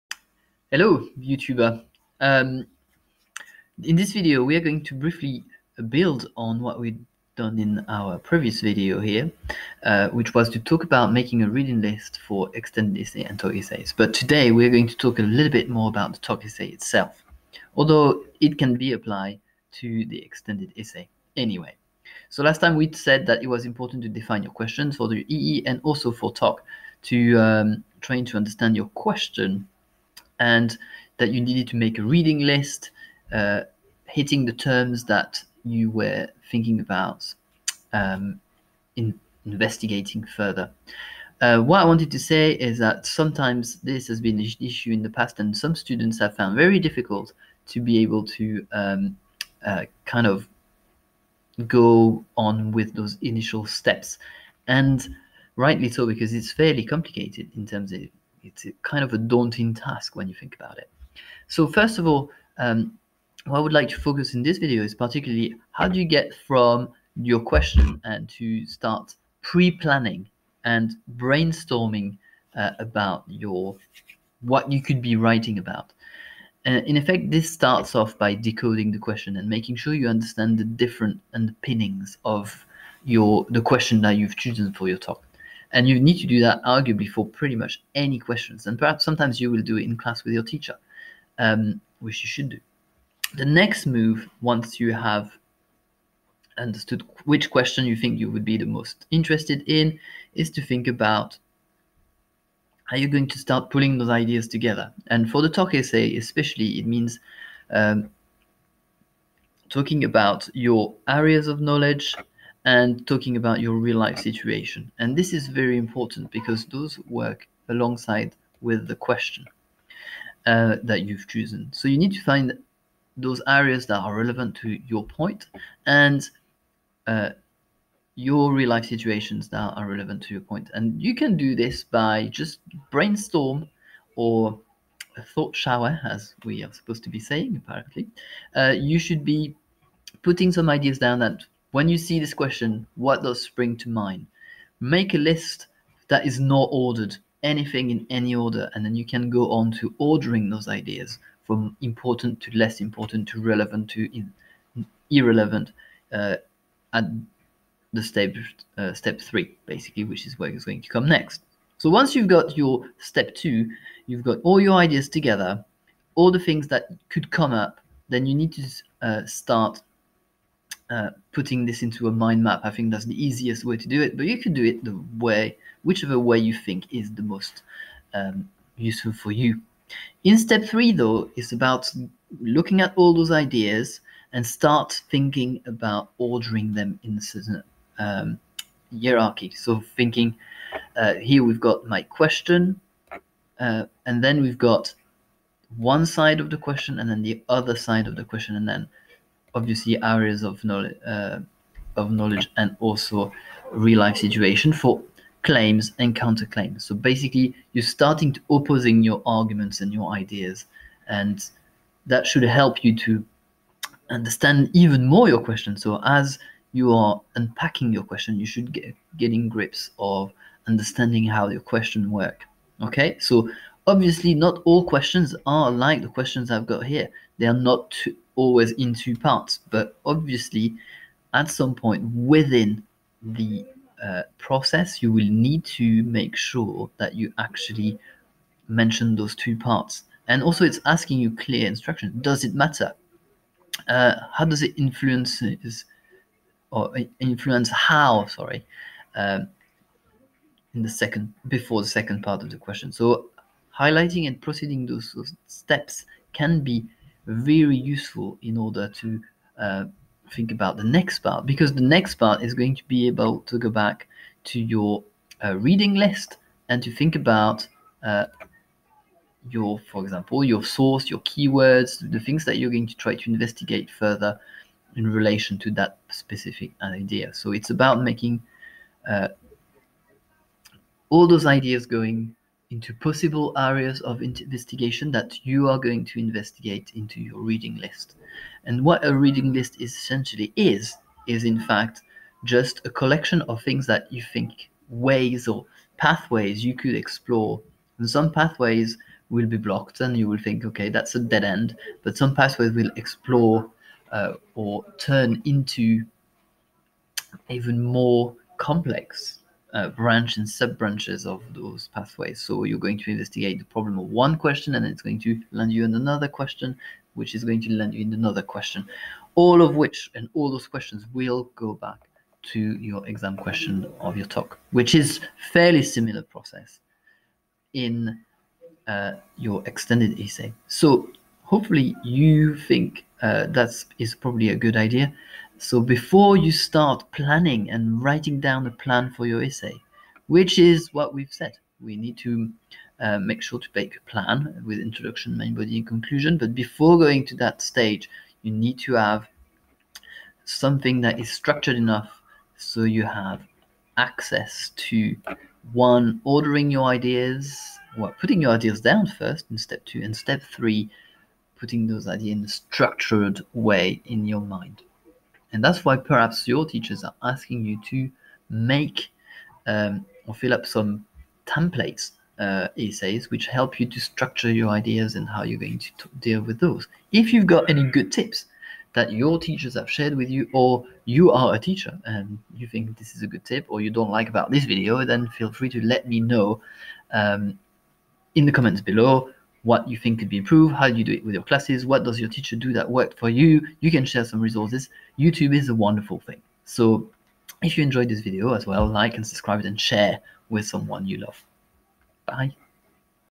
Extended pieces IB Ep2: TOK unpacking your question , thinking, planning your Essay like a boss!
This video is about how you go about analysing your TOK essay question and how you can use some simple strategies such as brainstorming or and planning to maximise the efficiency of your theory of knowledge essay

In this video, I am tackling the issues of going over the processes behind writing extended pieces of work for the International Baccalaureate Diploma.  I have broken down this into small units so they can be made into manageable steps in order to help students understand how to structures their theory of knowledge essays and extended essays